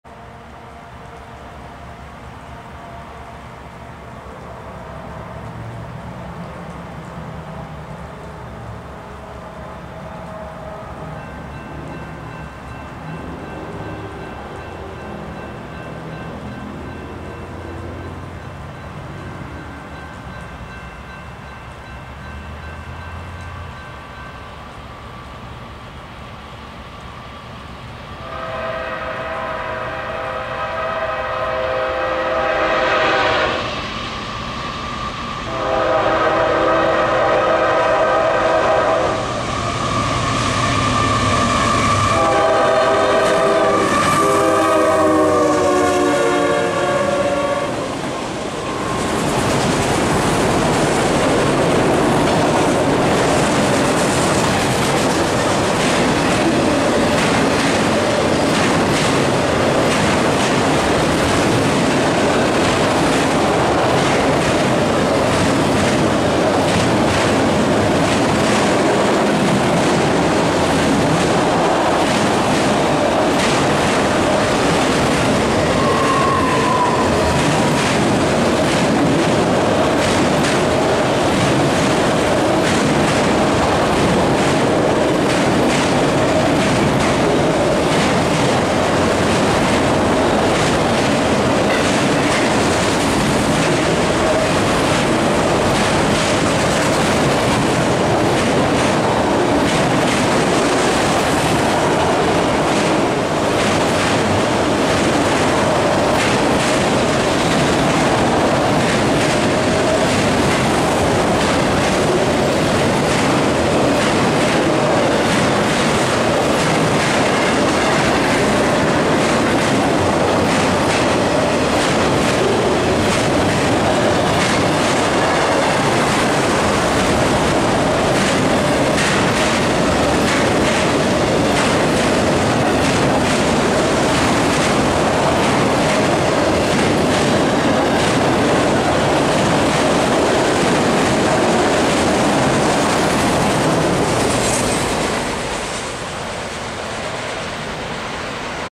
CSX AC44CW 590 w/ Sick K5LA Leads Empty Automotive Q210-15 on 1/15/22
A CSX AC44CW, BONGA, and Dash 8 lead empty automotive Q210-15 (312 axles, 7122ft) north on the Chattanooga Sub as the third of four straight northbounds in five hours. Unfortunately I couldn't get the Dash 8 leading, but I kinda like this sick K5LA. They would drop off 69 racks at Nissan and take the remaining four to Nashville instead of going to Louisville. There haven't been many pickups from Nissan lately due to the car microchip shortage.
Filmed at W Main St. in Murfreesboro, TN.

CSX 590- AC44CW/CW44AH (Sick NF K5LA w/ dead 3A)